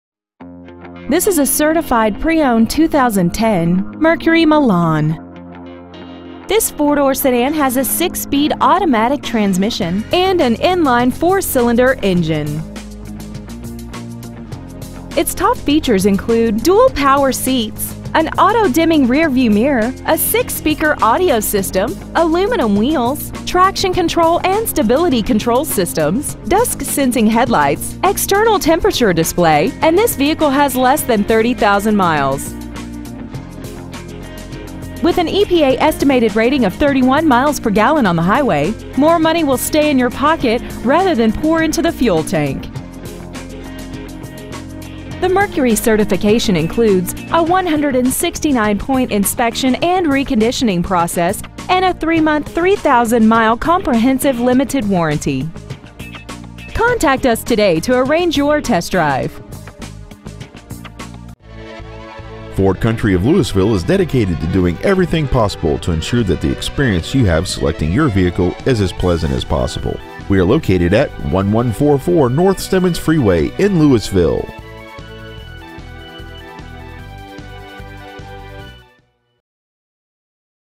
Certified 2010 MERCURY MILAN TX
Year : 2010
 Make : MERCURY
 Model : MILAN
 Engine : 2.5L I4
 Trans . : AUTO 6SPD
 Exterior : SMOKESTONE METALLIC
 Miles : 29,245
 Interior : BLACK
 Stock : P4503